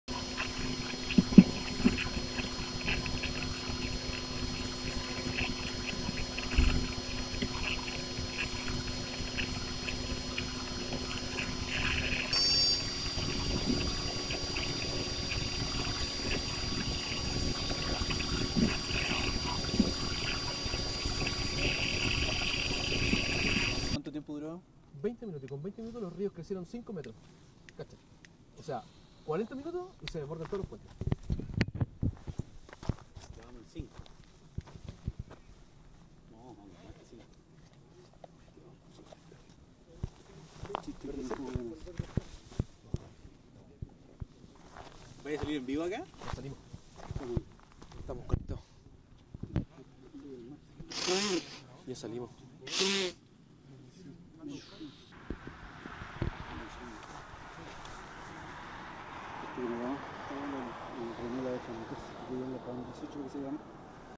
Lightning and lava: Chile volcano violently explodes in electric storm
Chile's volcano Calbuco spews smoke and ash for the first time in more than 50 years, forcing evacuations. 23 April 2015.